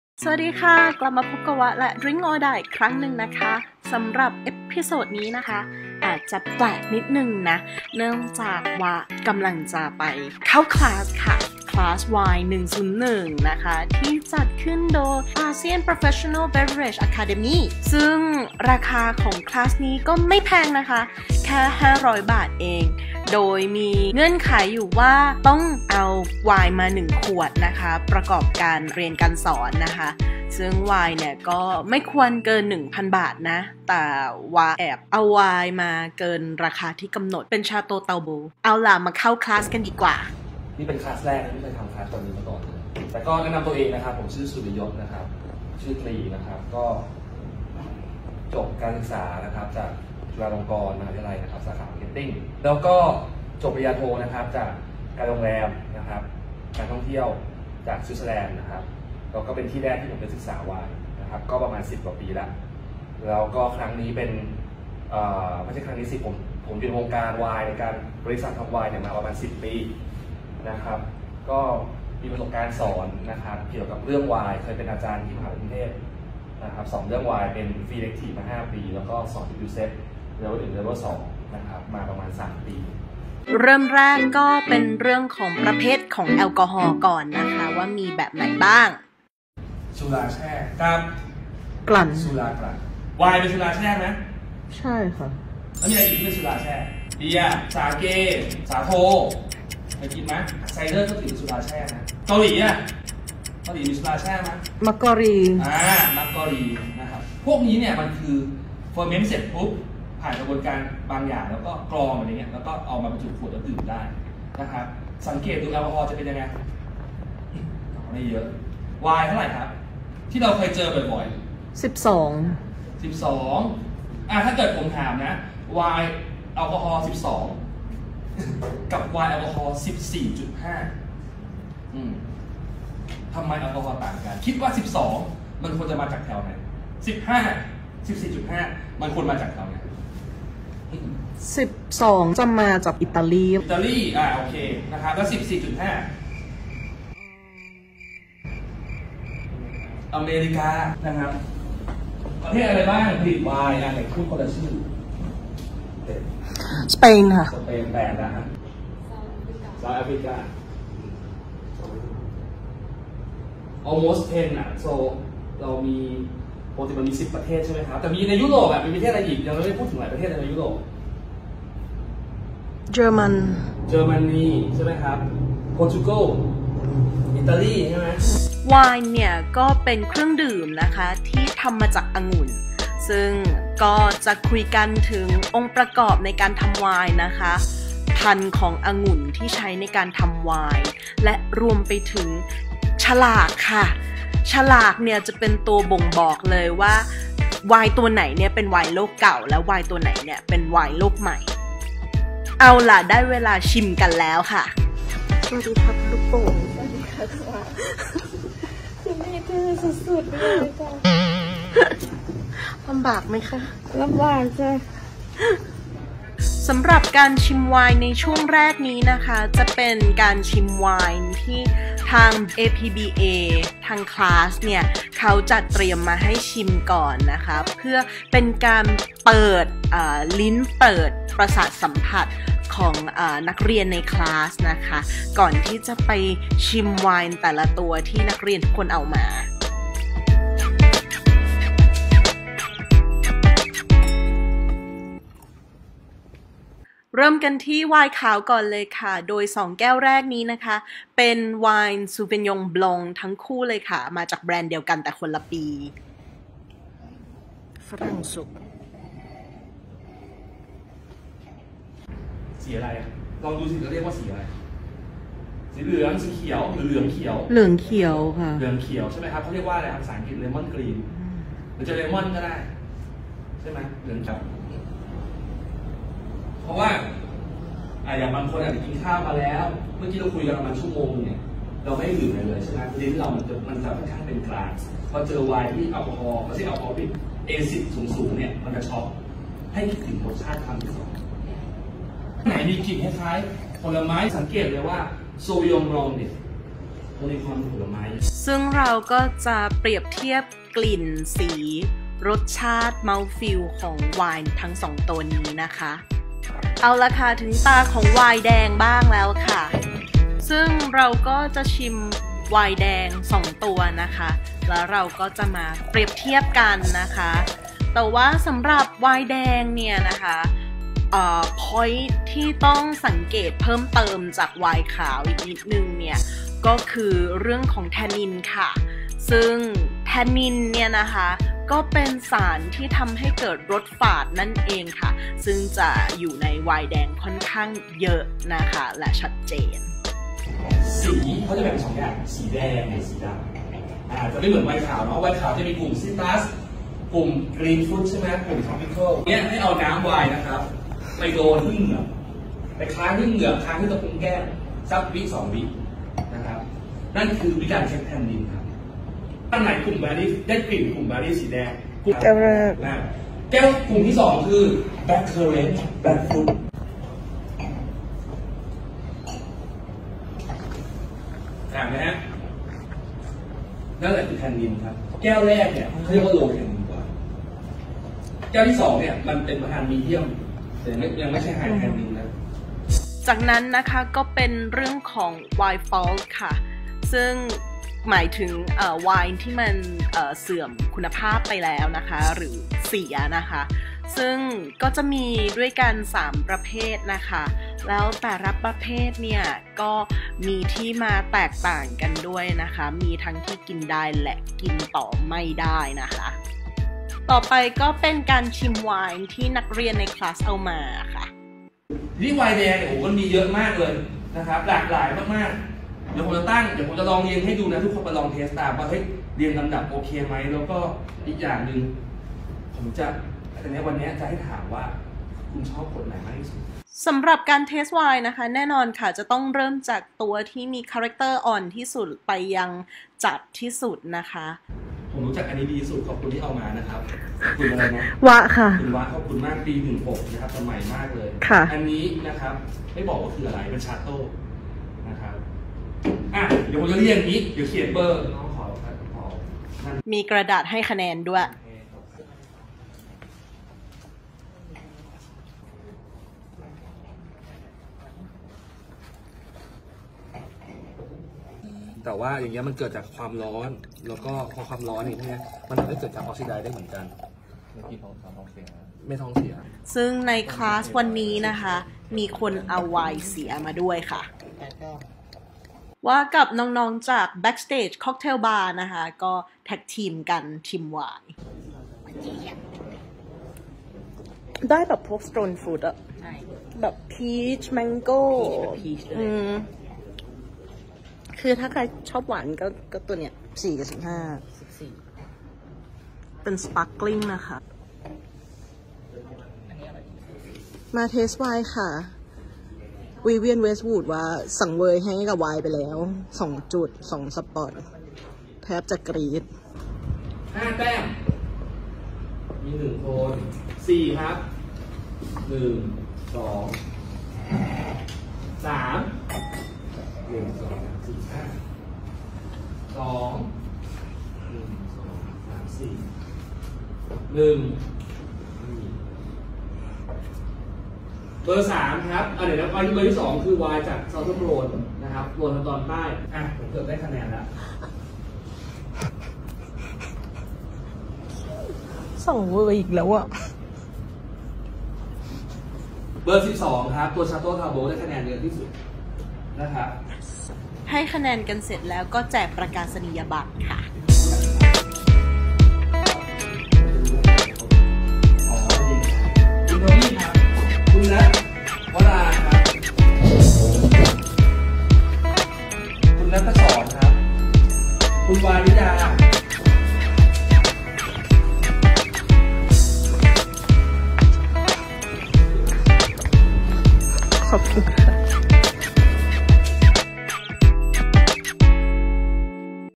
Class: Wine 101
I began to enjoy wine but I don't really know much about it so I decided to take a class, Wine 101, hosted by ASEAN Professional Beverage Academy.
Well, let's find out how did I do as a student.
for contributing a package of cruelty-free false eyelashes to complete my look before going out.